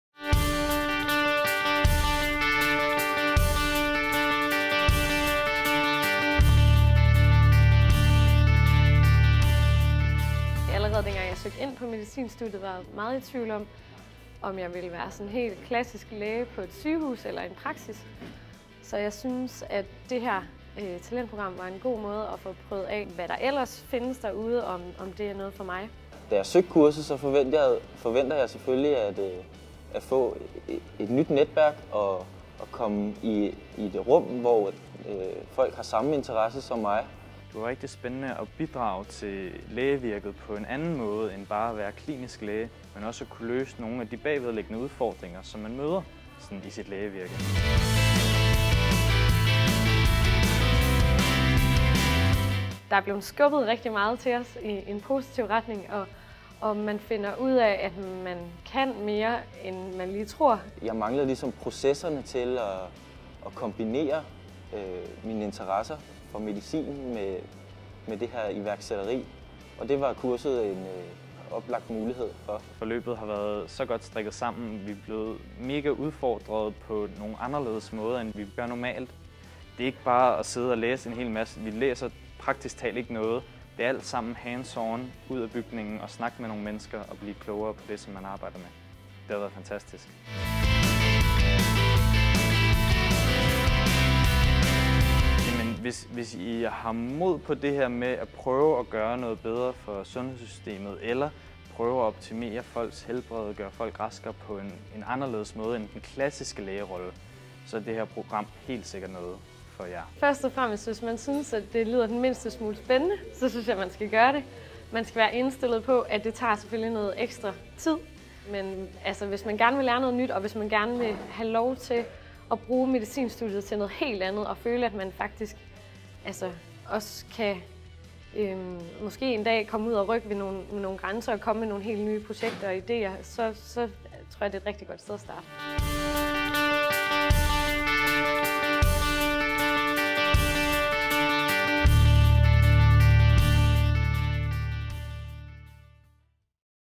Erhversrettet talentforløb på AU Health
Det erhvervsrettede talentforløb er et tilbud målrettet medicinstuderende ved Health på Aarhus Universitet. Forløbet kombinerer de studerendes ordinære undervisning med studier i innovation og entreprenørskab, og orienterer sig mod nutidens behov i medicinalindustrien og sundhedssektoren.